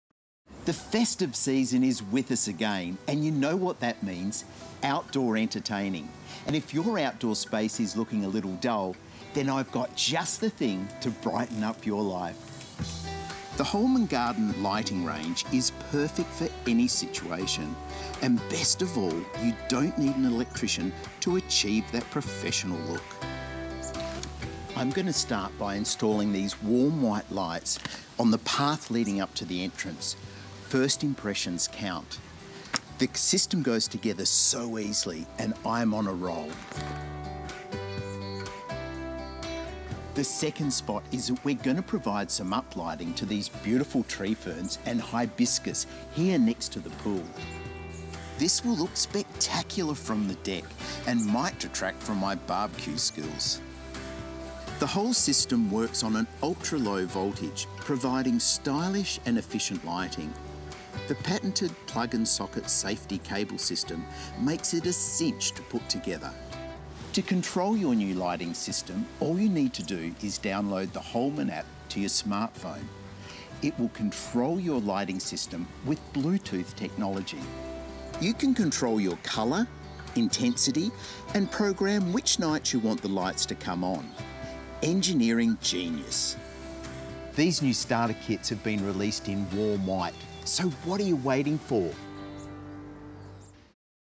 The Garden Gurus - Holman Garden Lighting
Holman offers an excellent outdoor lighting range, perfect to light up your entertainment areas for the coming festive season.